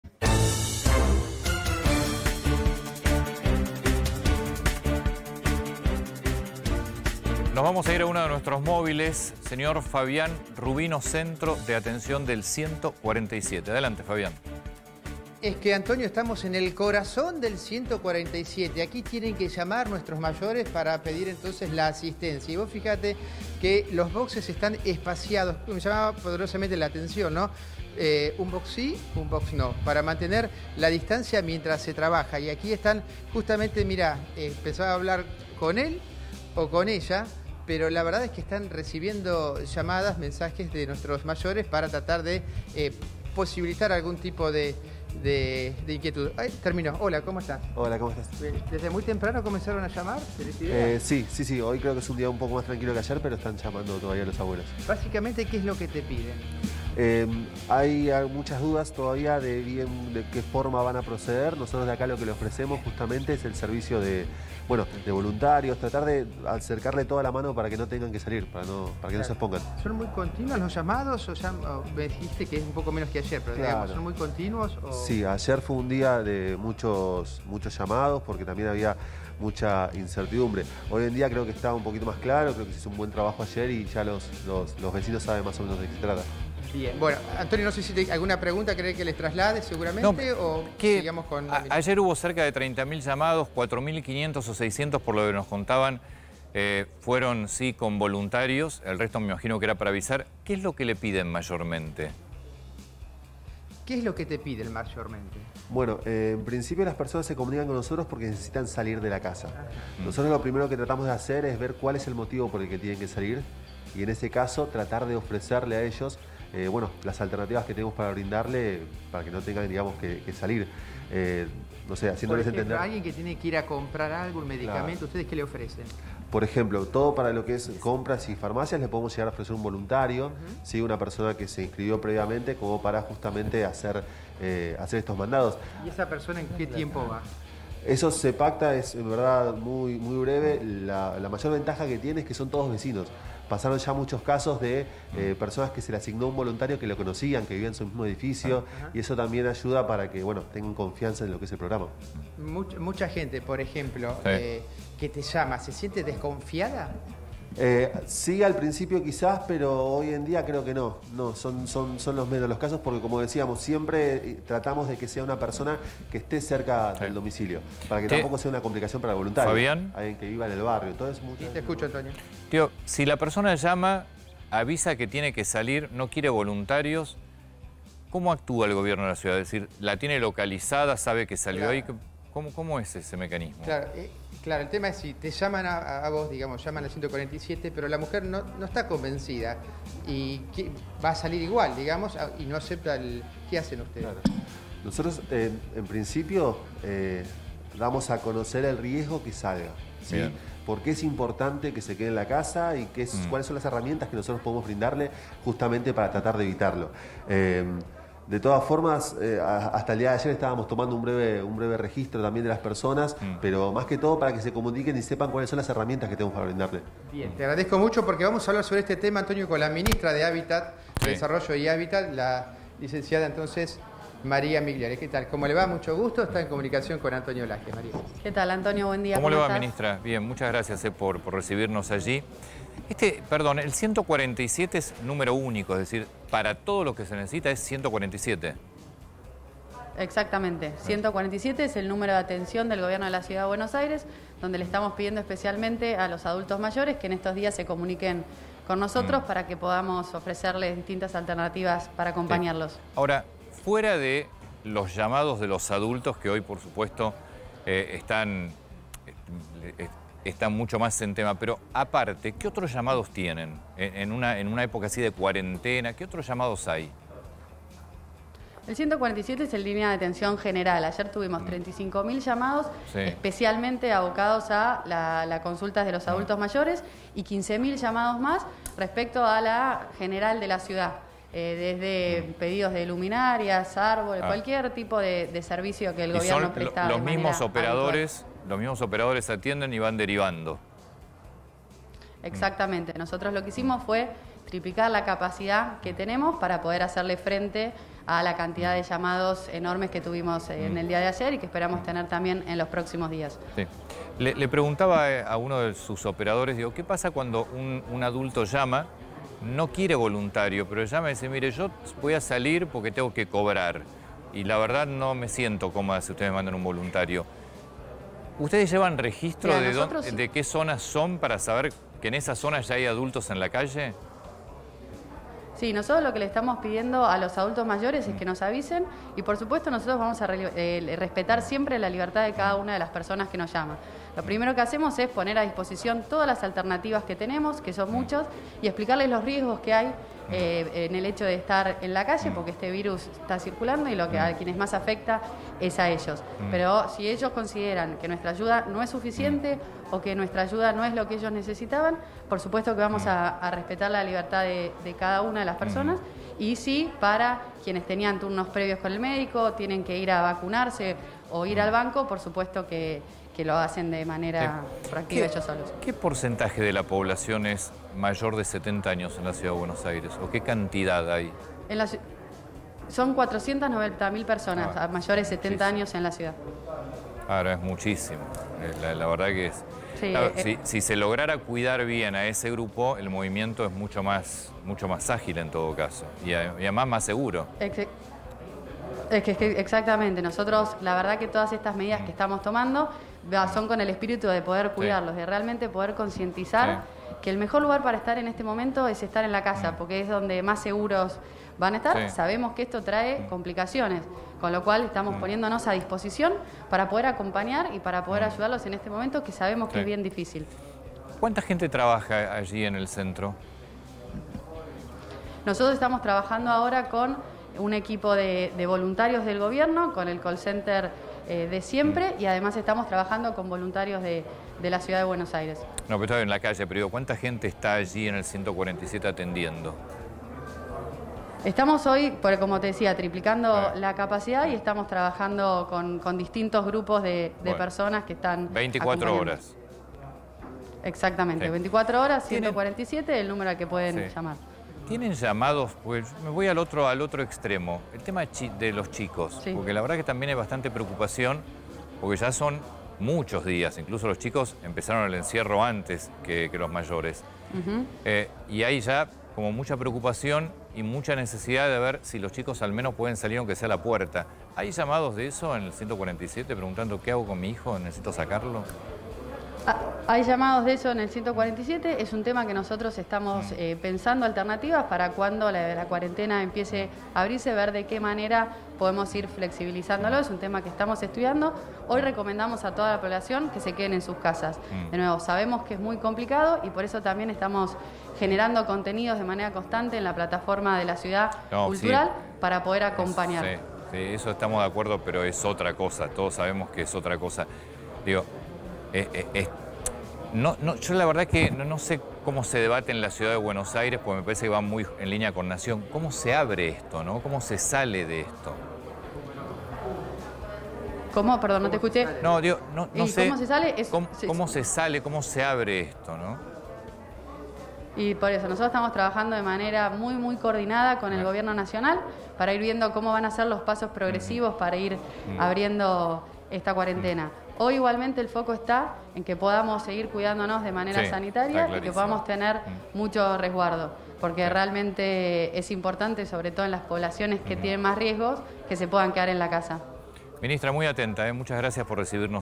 Así funciona la línea del 147 en el
@fabirubino1 te muestra cómo funciona la línea del 147 en el @gcba y habló con la ministra @MariaMigliore, sobre la atención a los adultos mayores por @A24COM